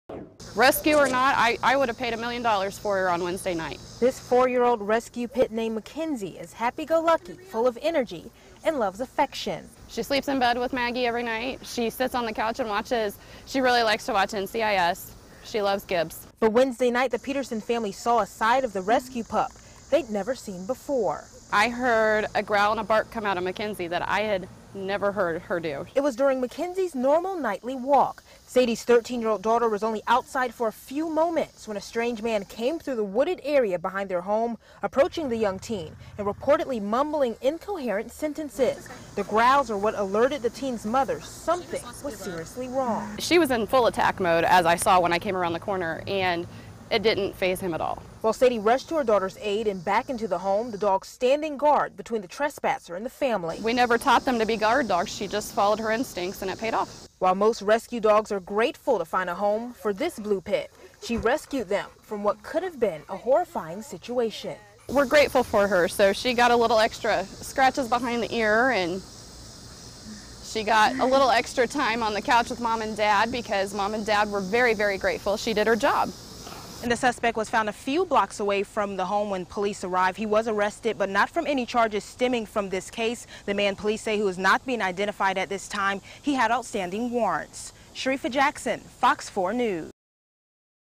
Rescue pup rescues 13-year-old from stranger
She was out for an evening walk with her dog when she was approached by a man with outstanding warrants. The mild-mannered dog sprang into action.